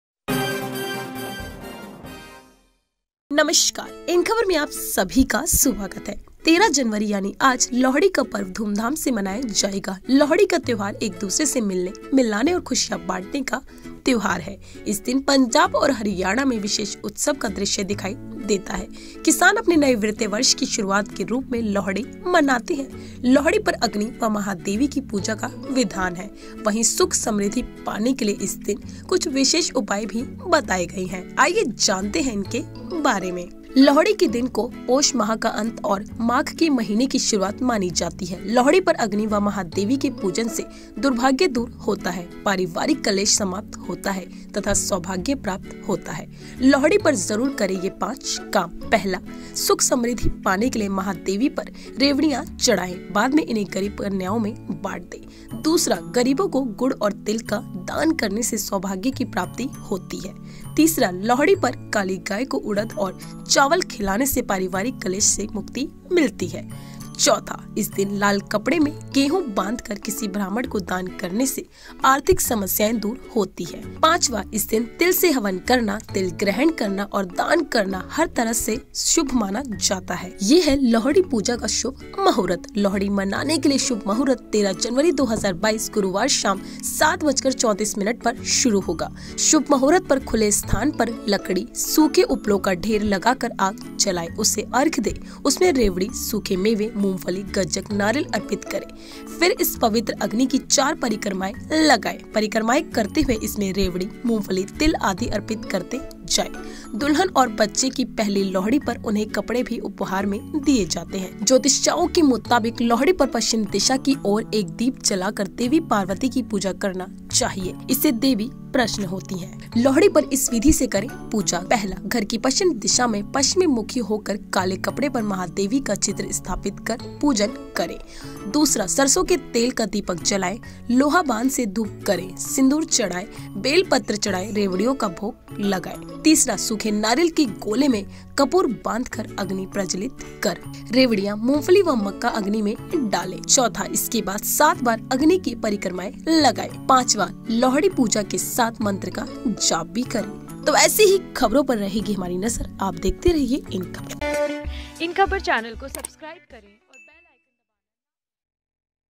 Lohri 2022: लोहड़ी पर सुख-समृद्धि पाने के लिए जरूर करें ये 5 काम, जानें पूजा विधि और शुभ मुहूर्त
Lohri 2022: लोहड़ी के मौके पर पंजाब में नई फसल काटी जाती है. इसलिए इन फसलों को किसान इष्ट देव के समर्पित करके सुख-समृद्धि की प्रार्थना करते हैं. लोहड़ी पर अग्नि व महादेवी की पूजा का विधान है. वहीं सुख-समृद्धि पाने के लिए इस दिन कुछ विशेष उपाय भी बताए गए हैं.